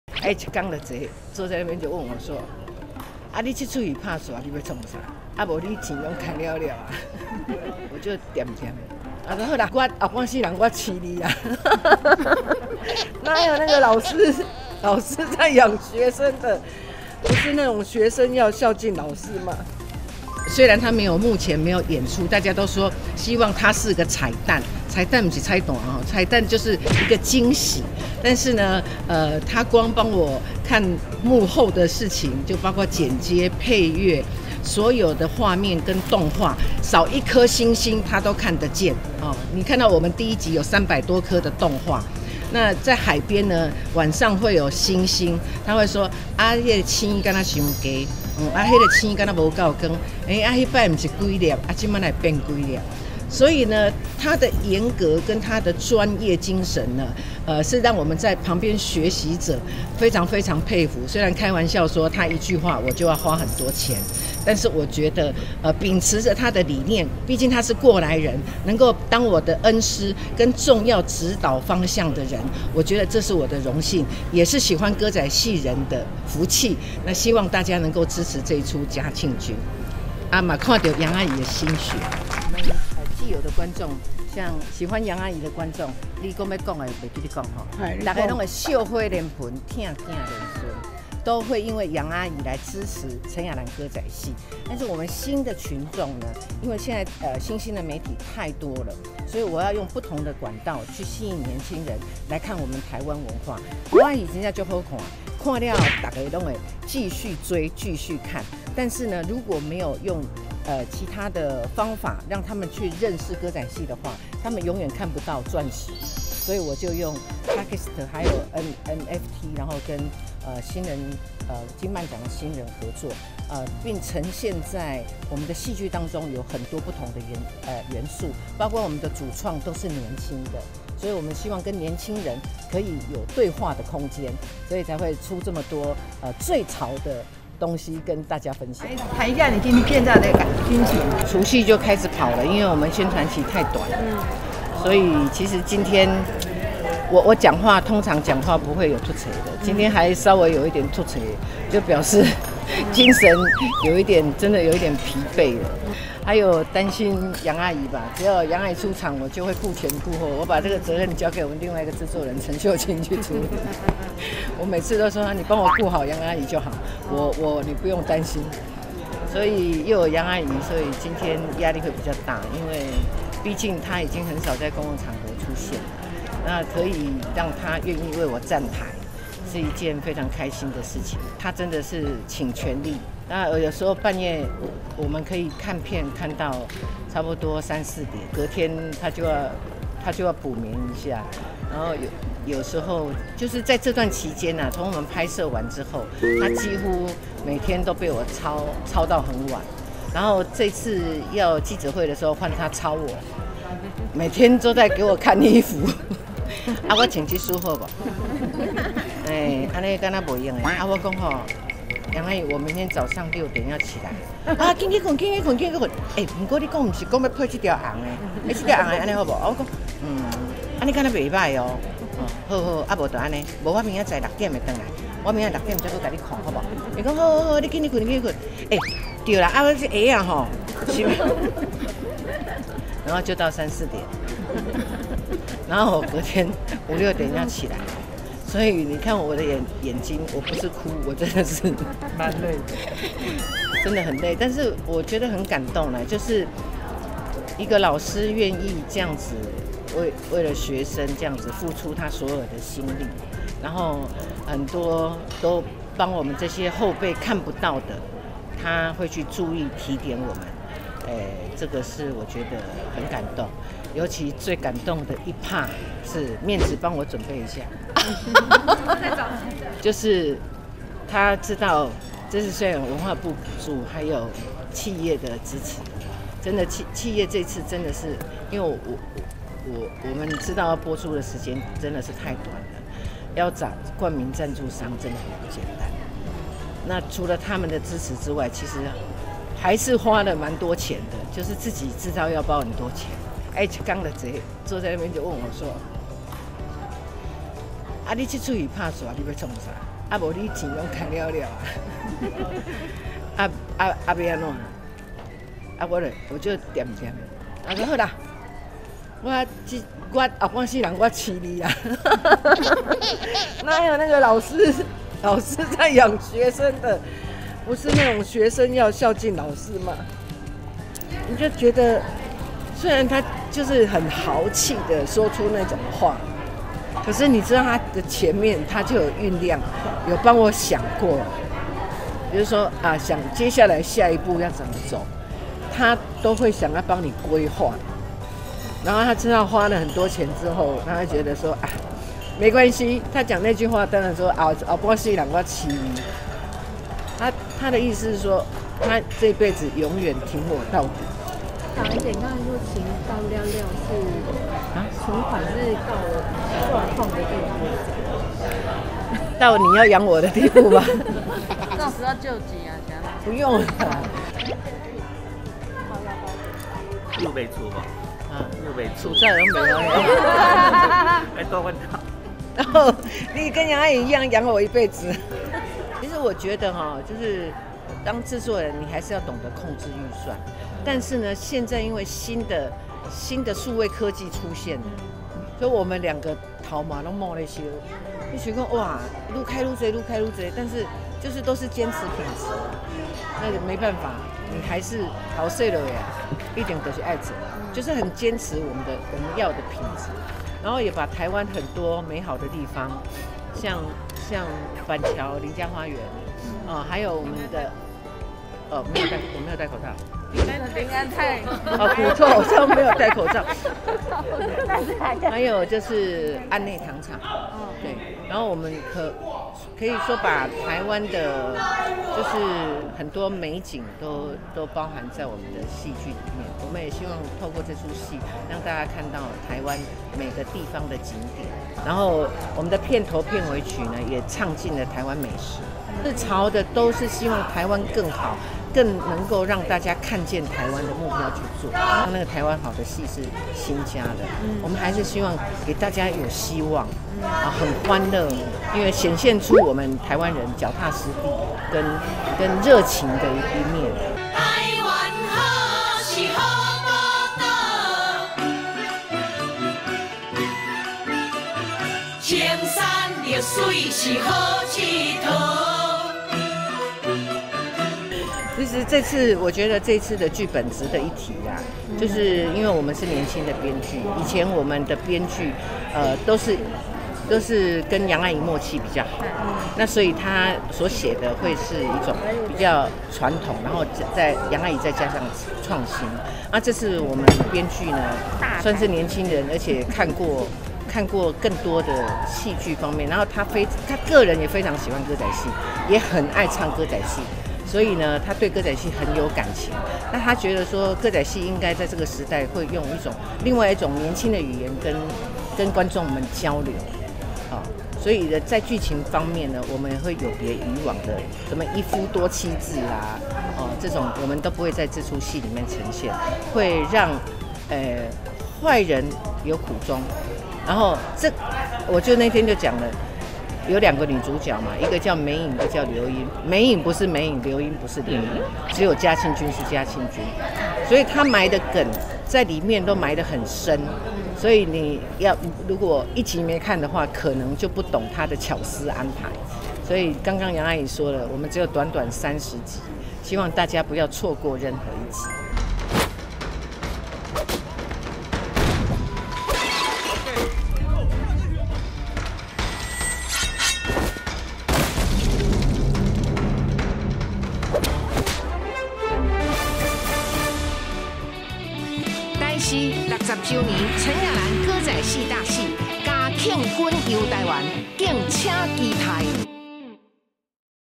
楊麗花這句話讓陳亞蘭淚崩！最潮歌仔戲《嘉慶君遊臺灣》梗點滿滿
歌仔戲天王小生陳亞蘭打造最潮歌仔戲《嘉慶君遊臺灣》集結八大看點，製作上斥資百萬棚內打造嘉慶君橫渡黑水溝大船、透過飽覽臺灣風光廣場舞歌頌臺灣好、打造歌仔戲最大閃光彈，由她與陳怡真這對經典CP再配對獻吻戲，以及與台北時裝週設計師王彥熙量身打造時尚戲服。